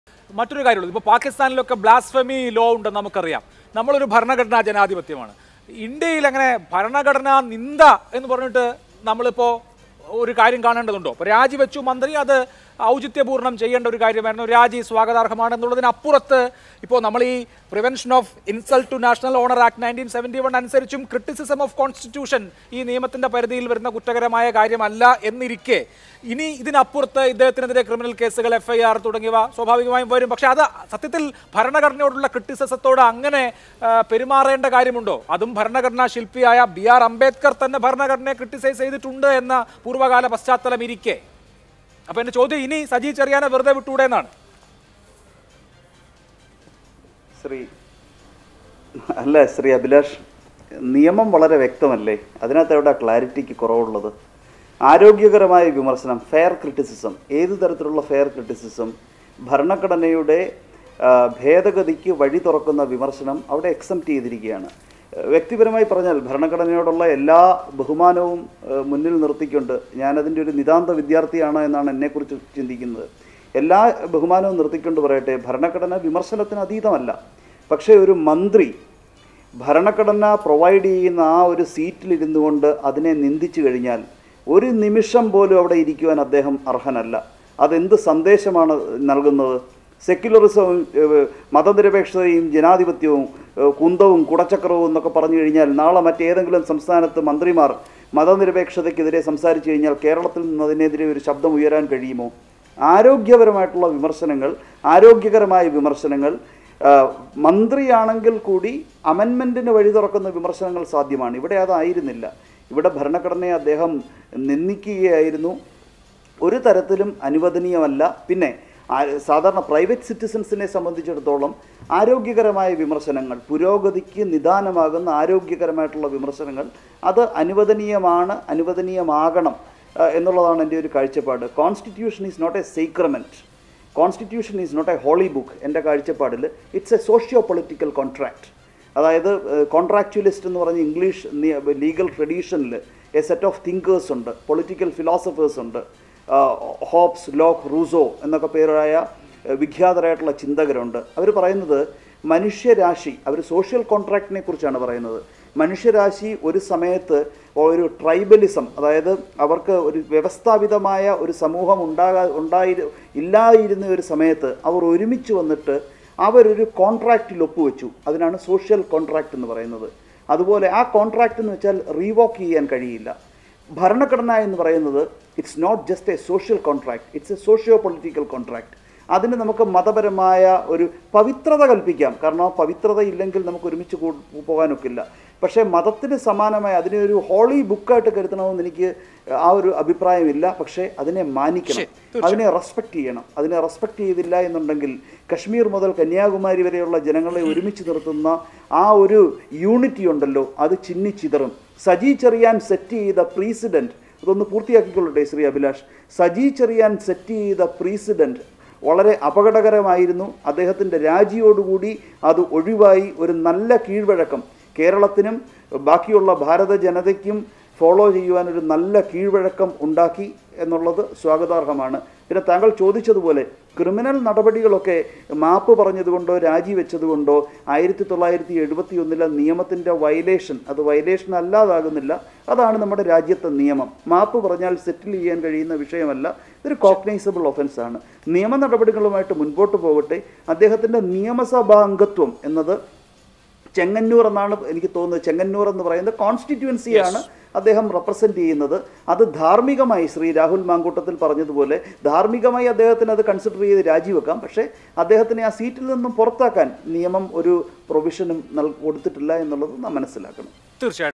'മതനിരപേക്ഷതയും കുന്തവും കുടച്ചക്രവുമെന്ന് പറഞ്ഞ് ഭരണഘടനയെ നിന്ദിക്കുകയായിരുന്നു' - അഡ്വ MR അഭിലാഷ്
ഭരണഘടന നൽകുന്ന സീറ്റിലിരുന്ന് ഒരു മന്ത്രി അതിനെ നിന്ദിച്ചു കഴിഞ്ഞാൽ ഒരു നിമിഷം പോലും അവിടെ ഇരിക്കാൻ അർഹതയില്ലെന്നും, മതനിരപേക്ഷതയും കുന്തവും കുടച്ചക്രവുമെന്ന് പറഞ്ഞ് ഭരണഘടനയെ നിന്ദിക്കുകയായിരുന്നുവെന്നും അഡ്വ എം ആർ അഭിലാഷ്

Happy viewing!